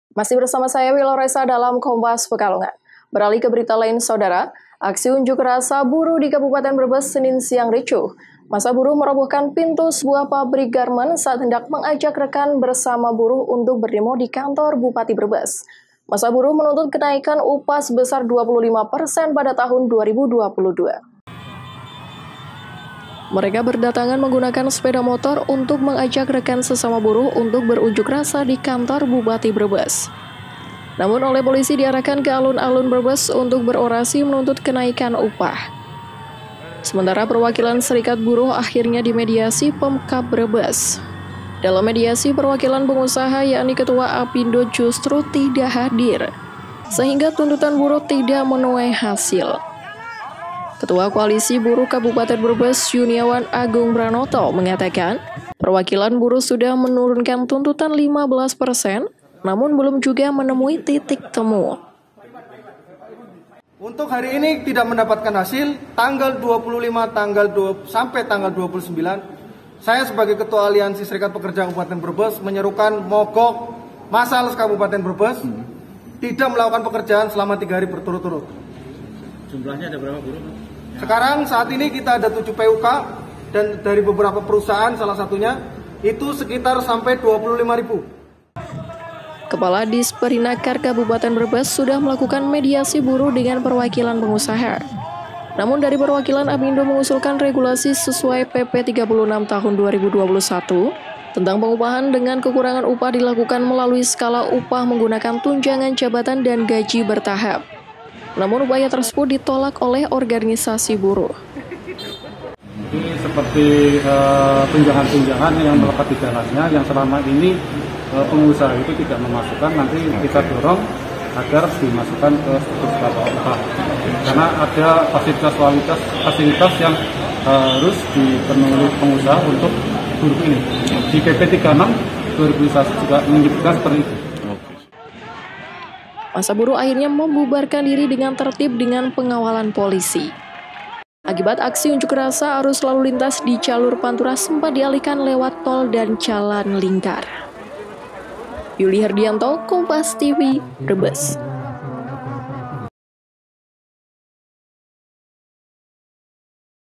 Demo Buruh Tuntut Kenaikan Upah
PEKALONGAN, KOMPAS.TV - Ribuan buruh dari berbagai pabrik di Kabupaten Brebes, mendatangi salah satu pabrik garmen di Kecamatan Kersana. Mereka berdatangan menggunakan sepeda motor untuk mengajak rekan sesama buruh untuk berunjuk rasa di kantor Bupati Brebes.

Namun oleh polisi diarahkan ke alun-alun Brebes untuk berorasi menuntut kenaikan upah. Sejumlah perwakilan serikat buruh akhirnya dimediasi Pemkab Brebes.

Dalam mediasi perwakilan pengusaha yakni ketua apindo justru tidak hadir, sehingga tuntutan buruh tidak menuai hasil. Ketua koalisi buruh Kabupaten Brebes, Yuniawan Agung Pranoto mengatakan perwakilan buruh sudah menurunkan tuntutan 15 persen namun belum juga menemui titik temu.

Sejumlah organisasi buruh mengancam mogok kerja selama tiga hari pada 26 hingga 29 november mendatang, karena aksi tuntutan mereka tidak dipenuhi. Mereka tetap menuntut kenaikan upah buruh tahun 2022 setidaknya naik 10 persen dibanding tahun sebelumnya.

Kepala disperinaker Kabupaten Brebes, sudah melakukan mediasi buruh dengan perwakilan pengusaha. Namun dari perwakilan apindo mengusulkan regulasi sesuai PP 36 tahun 2021 tentang pengupahan dengan kekurangan upah dilakukan melalui skala upah menggunakan tunjangan jabatan dan gaji bertahap. Namun upaya tersebut ditolak oleh organisasi buruh.

Seperti diketahui upah minimum Kabupaten Brebes tahun 2022 telah ditetapkan yakni sebesar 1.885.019 juta rupiah atau naik 0,97 persen dari UMK 2021. Namun kenaikan ini menurut buruh masih sangat kecil. Massa buruh akhirnya membubarkan diri dengan tertib dengan pengawalan polisi. Akibat aksi unjuk rasa arus lalu lintas di jalur pantura sempat dialihkan lewat tol dan jalan lingkar.